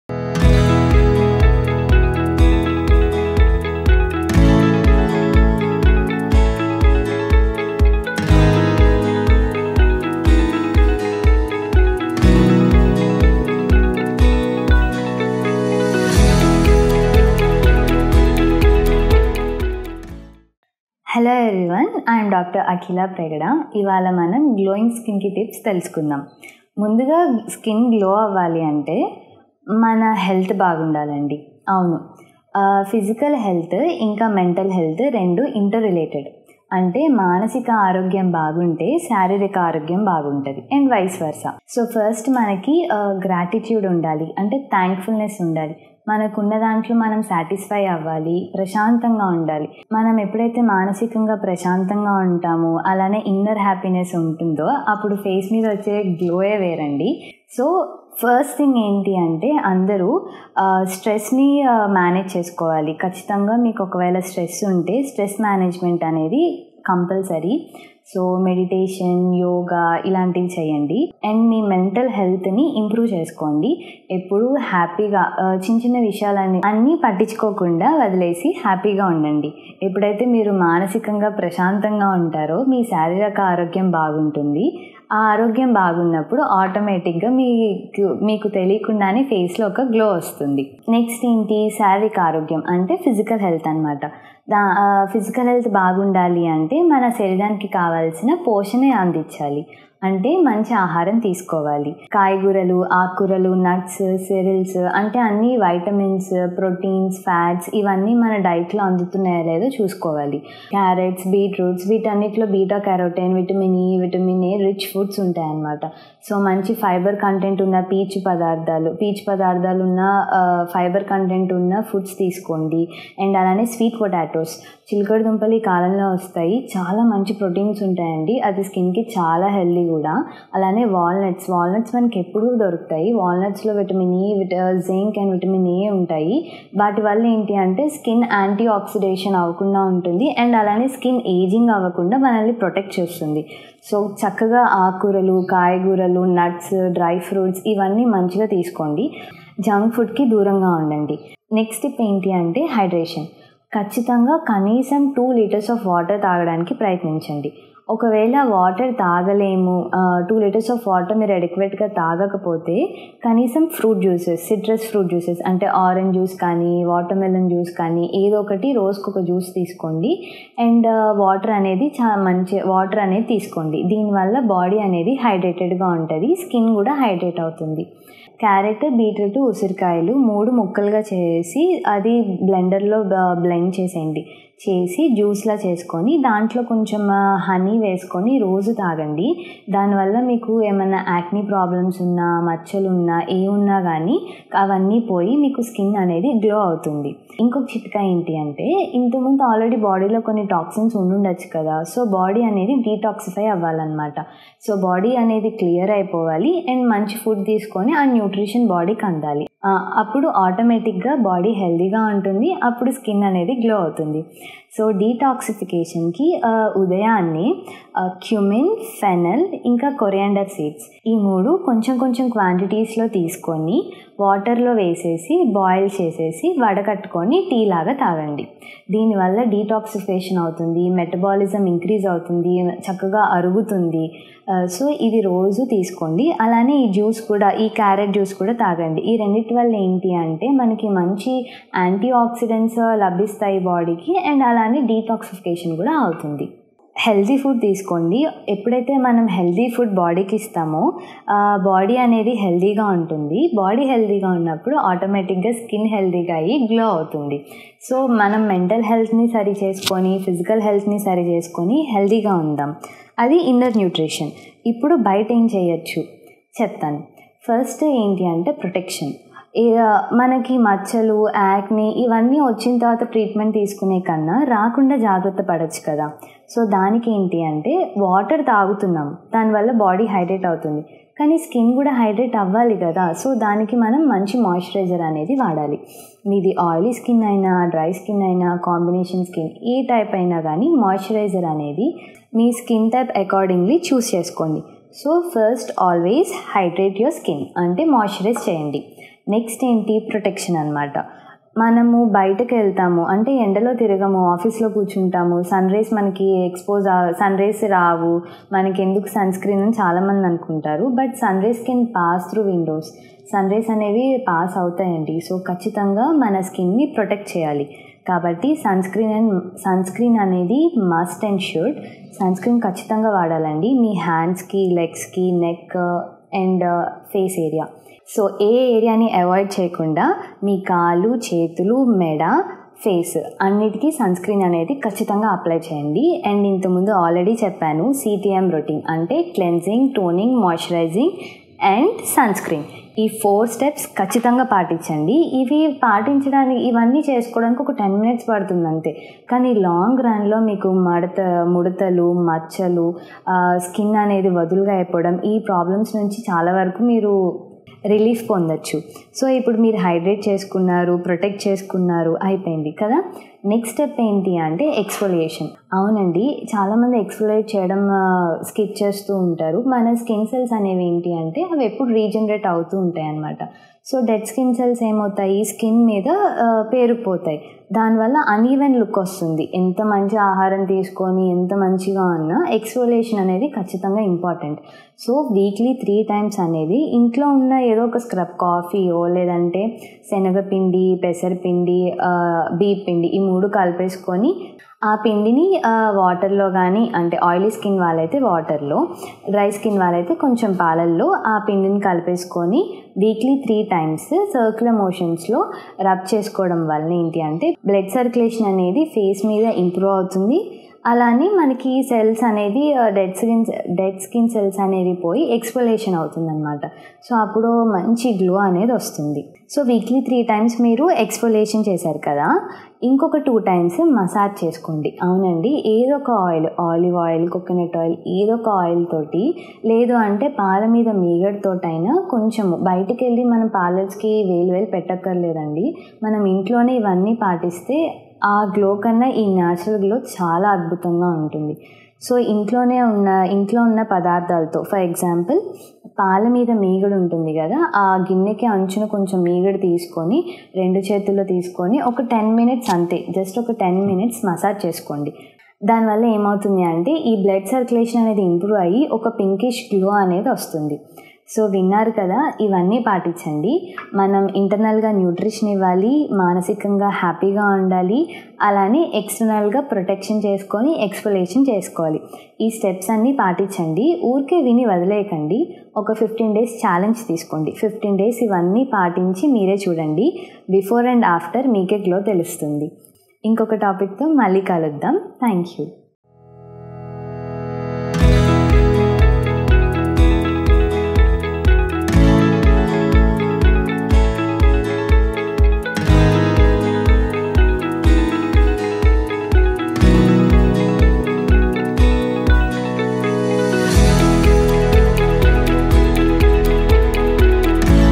Glowing Skin Care Tips in Telugu | Natural Home Remedies | Beautiful Skin | Dr. Akhila
Description : Simple Natural Remedies for Glowing Skin Care Routine in Telugu By Dr. Akhila from Dondapati Wellness Clinic, Vijayawadda